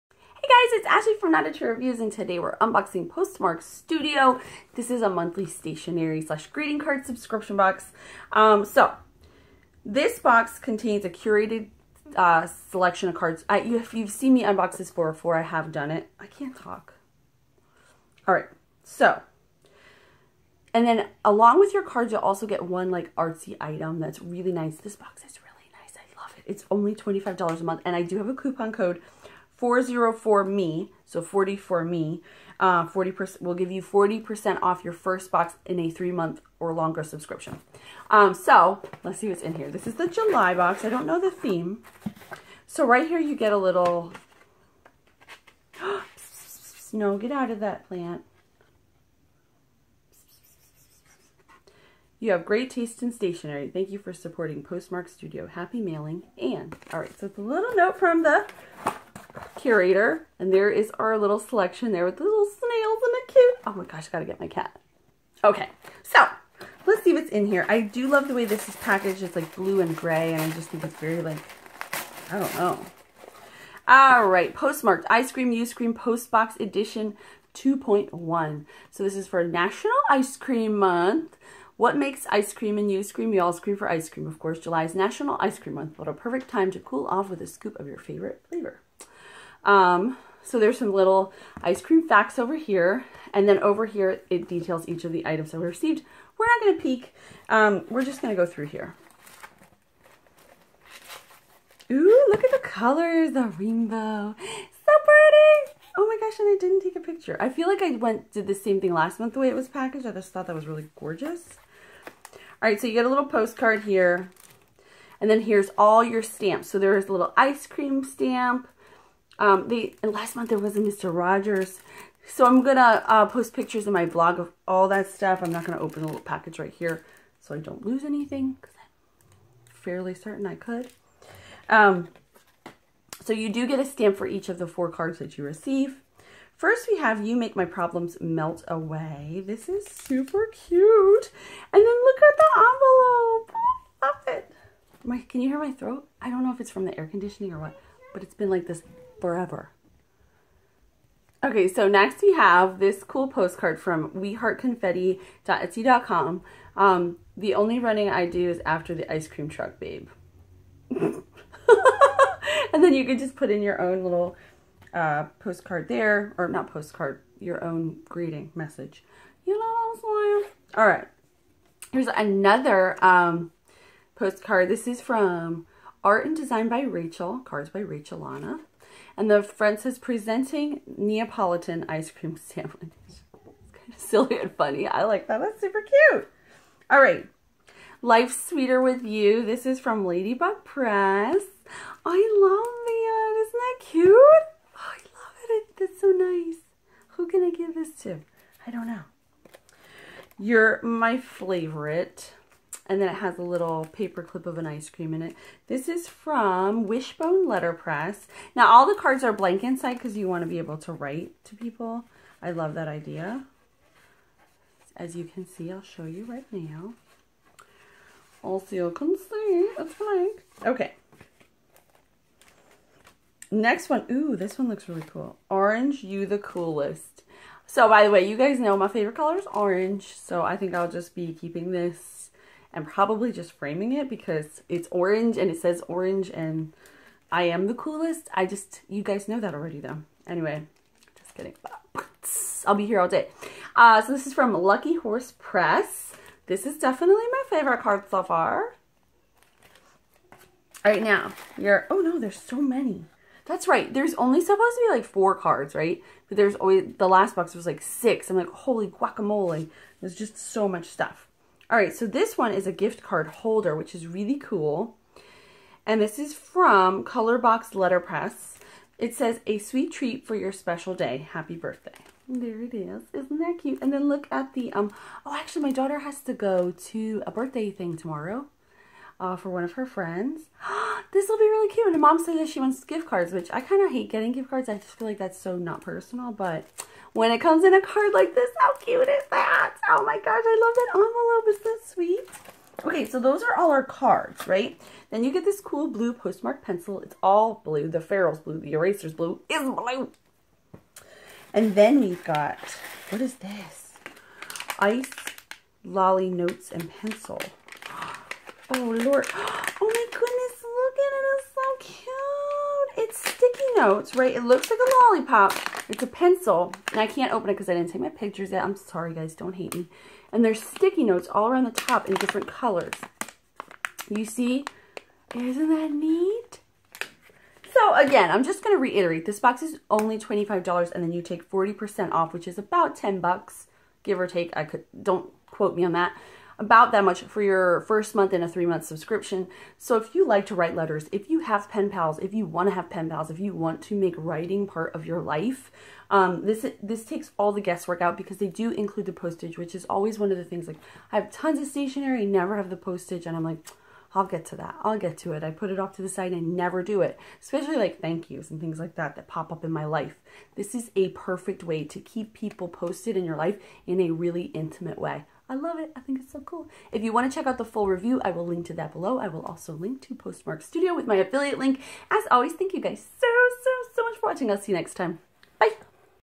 Postmark'd Studio Subscription Box Unboxing | July 2018
Subscription Box Unboxing and Review for the Postmark'd Studio Subscription Box. Each package contains a themed assortment of 4 greeting cards, 1 postcard, postage and an artsy doodad. Subscriptions are $25 +shipping each month.

FTC: Product was received free for review. All opinions are my own.